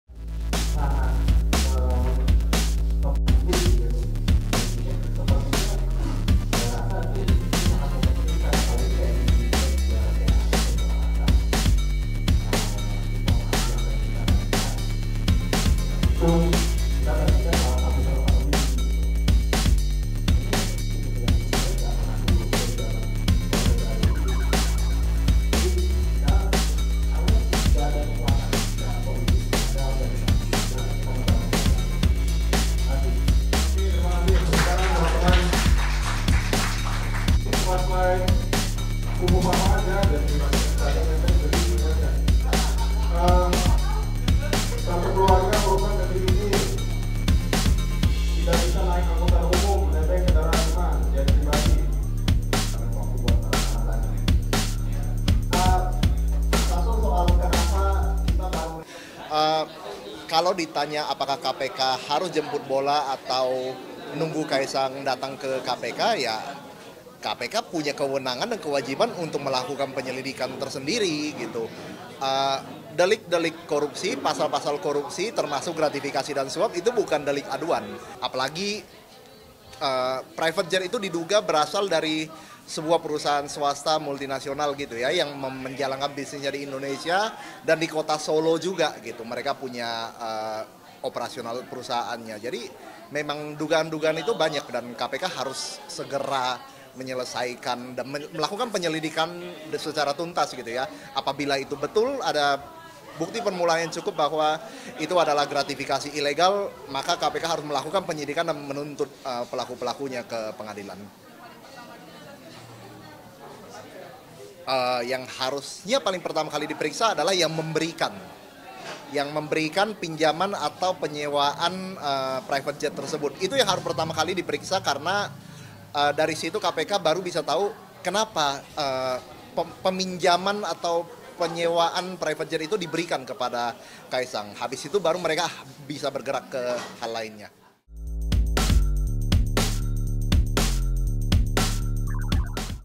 Soal Jet Pribadi Kaesang, Eks Penyidik Minta KPK Periksa Pemilik
Mantan penyidik KPK, Rieswin Rachwell menyatakan, KPK punya kewenangan dan kewajiban tersendiri melakukan penyelidikan terhadap dugaan gratifikasi jet pribadi kepada Kaesang Pangarep.

Meski Kaesang bukan pejabat negara, namun, patut diduga pemberian gratifikasi tersebut ada kaitan dengan keluarganya yang merupakan para penyelenggara negara, yaitu Jokowi sebagai Presiden, Gibran sebagai mantan Wali Kota Solo dan Bobby Nasution mantan Wali Kota Medan.

Pihak yang pertama diperiksa dalam kasus ini adalah pihak yamg memberikan pinjaman atau penyewaan jet pribadi tersebut.